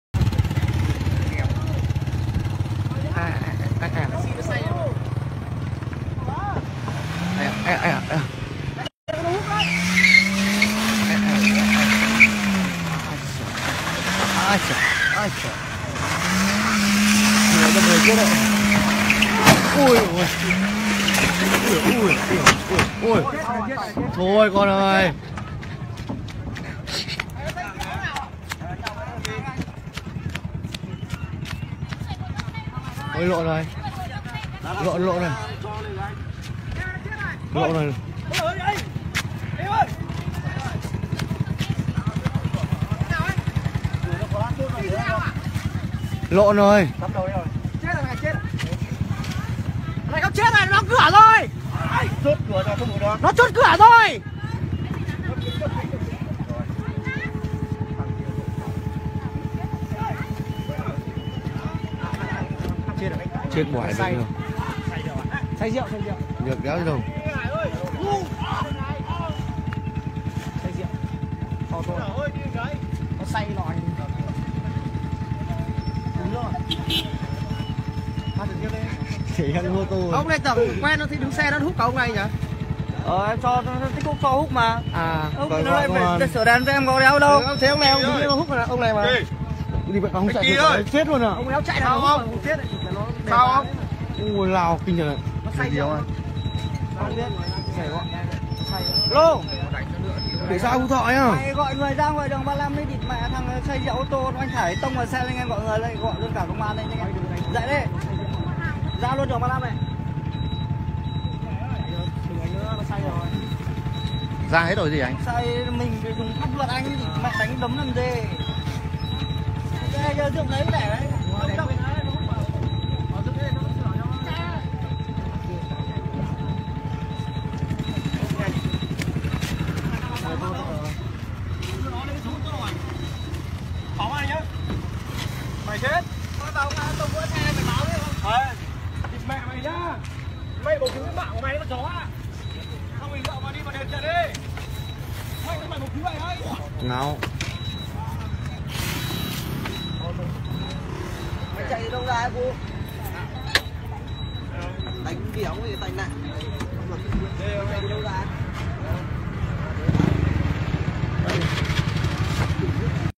Tài xế taxi phê rượu còn thích thể hiện và cái kết mát ruột mát gan
Sự việc xảy ra tại đoạn đường 35 thuộc thị xã Phú Thọ, tỉnh Phú Thọ.